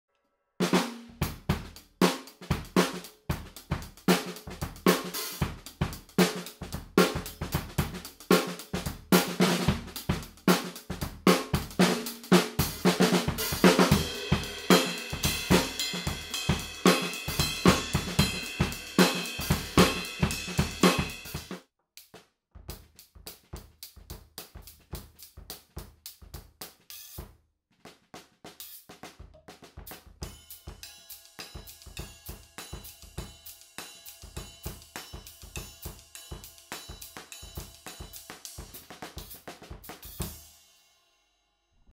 Low Volume Practice
Low Volume Practice with Zildjian L80s (14" and 18"), Aquarian Superpad 14" and Evans Soundoff on kick.
Zildjian K Custom Special Dry (non-reissue) 14" hats and 21" ride.
O'Neill Snare drum
Recorded on Zoom Q2N